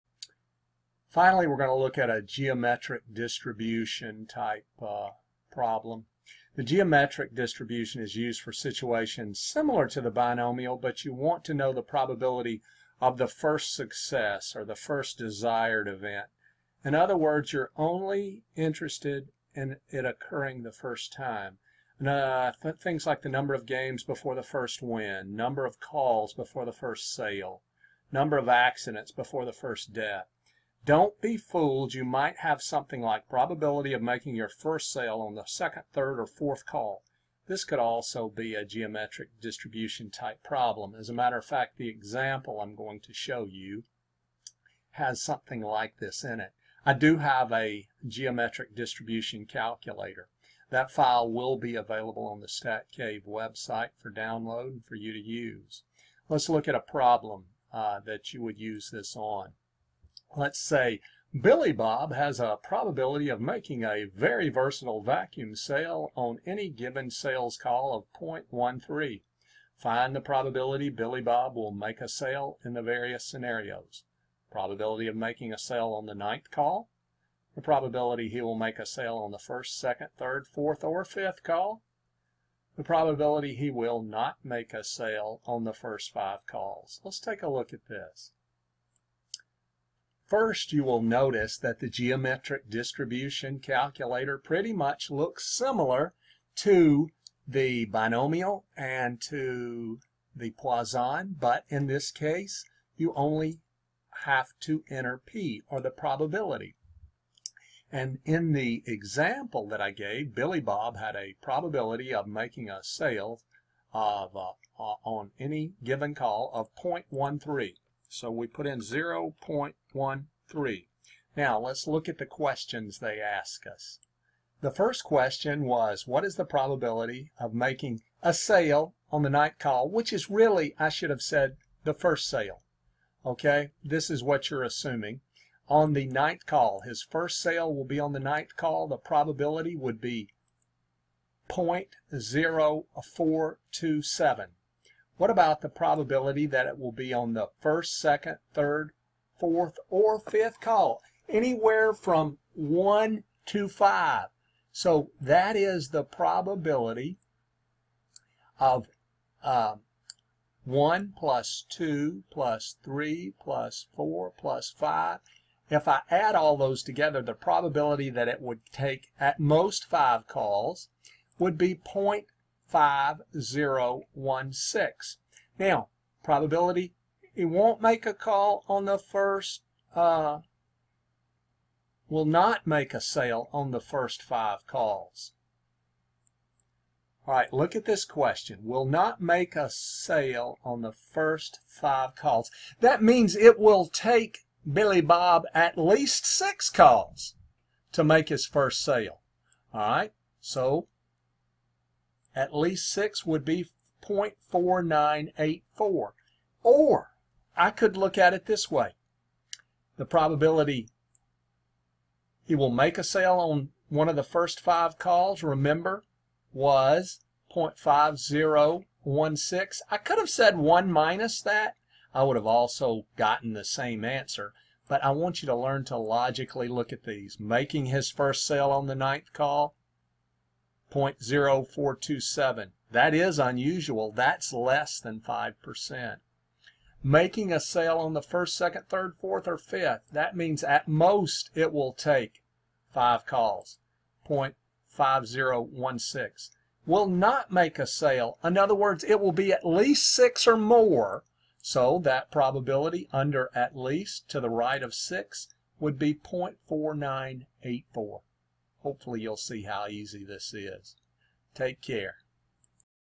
Geometric Distribution Problems with Excel
Math 221 Week 4 Geometric Distribution type problems. If you need the Excel file shown, you can find it/download it on the Stat Cave Blog